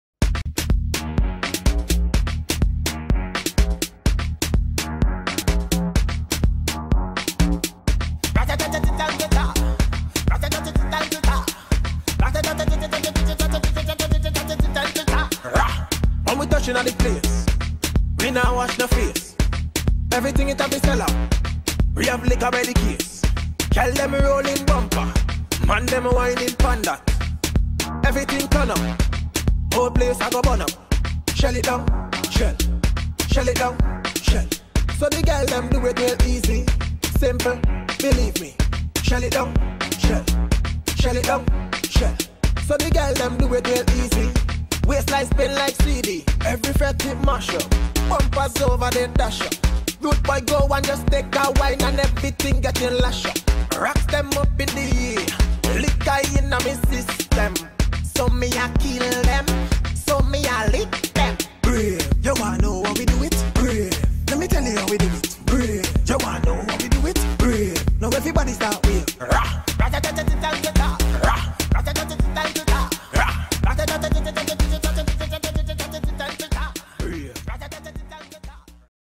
Inferno Band Launch 2018 - Hightlight
Inferno Carnival Band presents “Treasures of the Nile”, kicking off the 2018 Atlanta/Dekalb Carnival Band Launch Season!!!